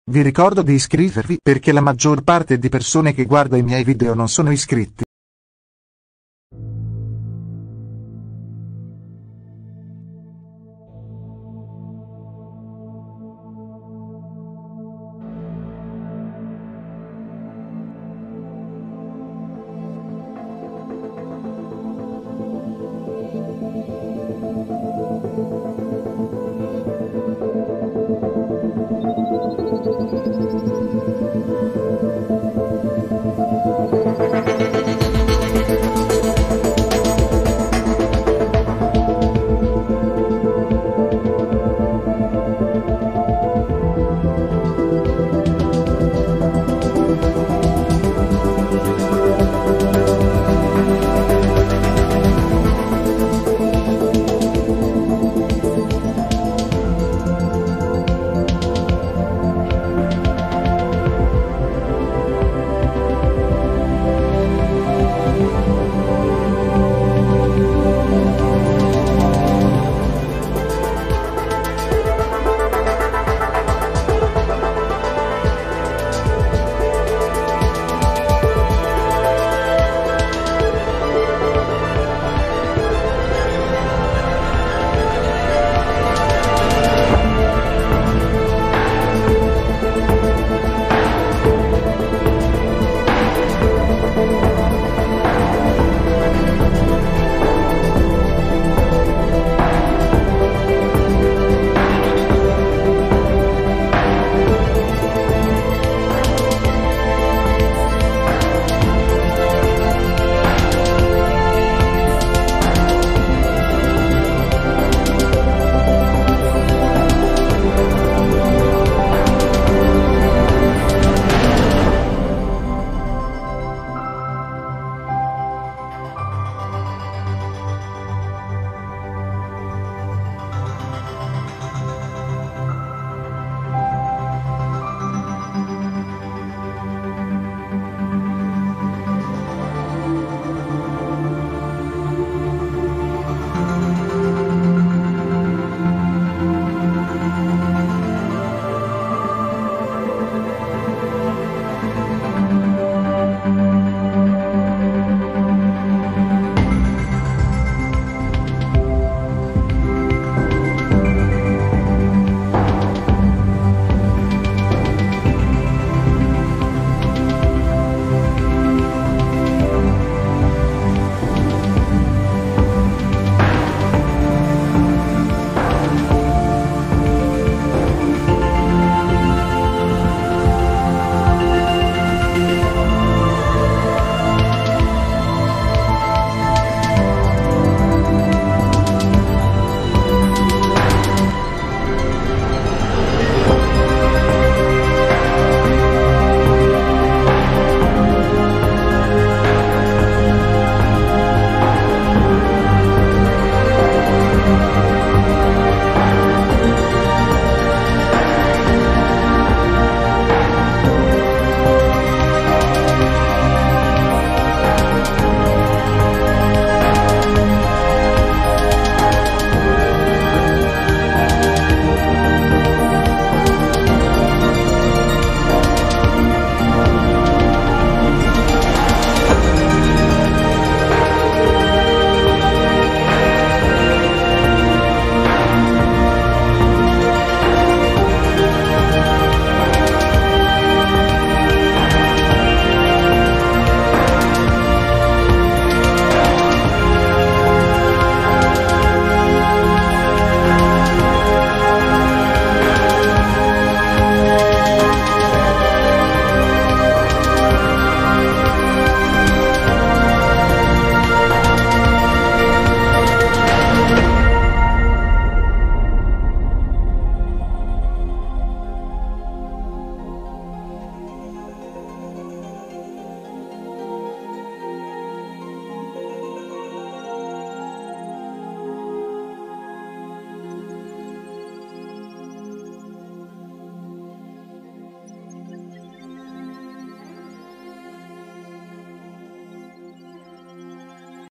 La situazione degli allenatori della Serie A italy
Ecco la situazione degli allenatori della nuova stagione di serie A TIM 2022/2023